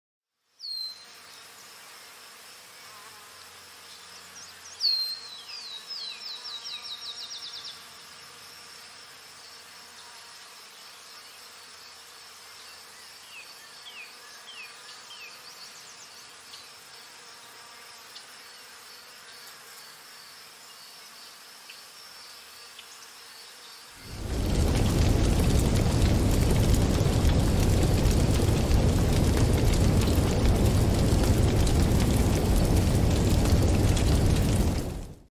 Brown Howler Monkey Alouatta guariba South America
Despite their namesake, Brown Howlers come in a variety of colours from buff to reddish orange and brown-black. They are well known for their expressive and loud howling which can be heard from up to 2 kilometres away. Excellent seed dispersers, they are essential for maintaining a healthy rainforest. Only 12% of their forest habitat remains. Protect them every time you shop and Boycottpalmoil